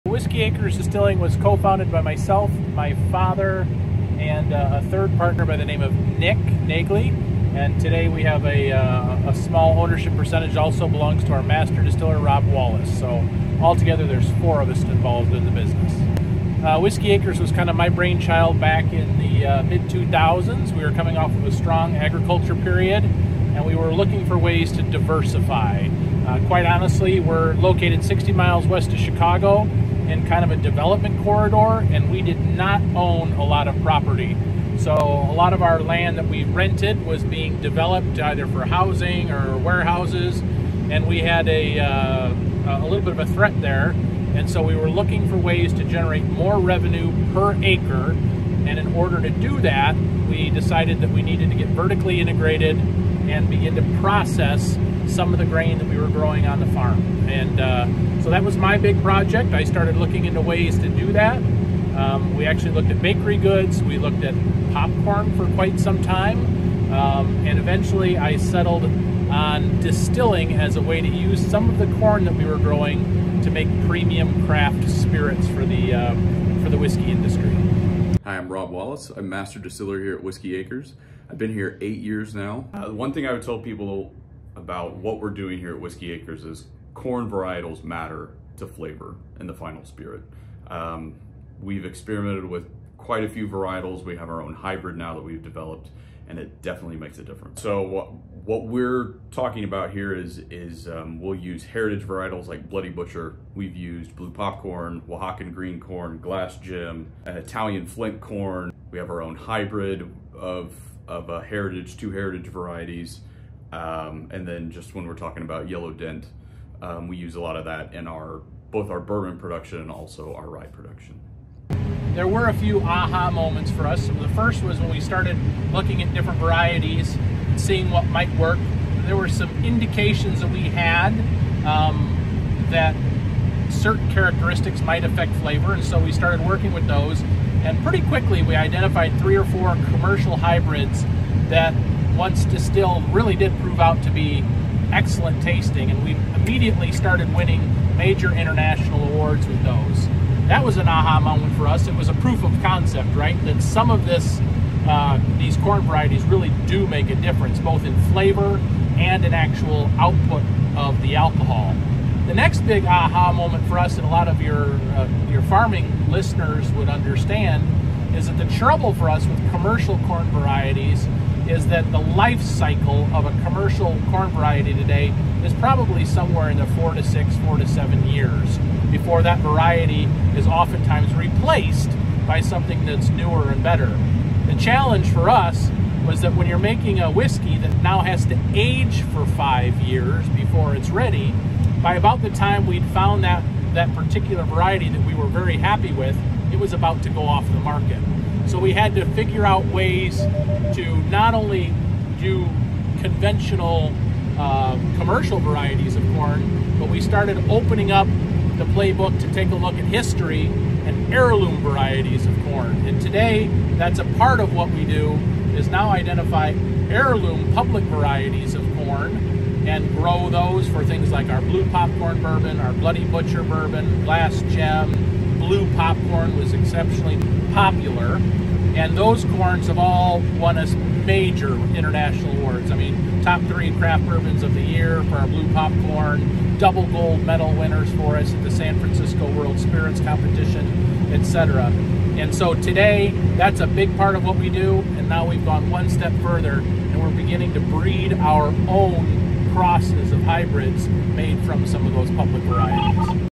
Whiskey Acres Distillery - From Corn to Whiskey
5th Generation farmer Jamie Walter and his father farm in Dekalb, Illinois.  About 20 years ago they were looking to diversify and landed on whiskey.  They grow the corn in their area looking at seed and even creating their own hybrid to bring award winning whiskey to Illinois.  @whiskeyacresdistillingco

Whiskey Acres was the winner of this years Illinois’ Top Craft Whiskey at the 2023 Heartland Whiskey Competition.

00:00 Intro
00:16 Why Whiskey Acres Started
01:17 Meet Rob Wallace - Master Distiller
02:02 Working with corn & whiskey - the AH-HA moments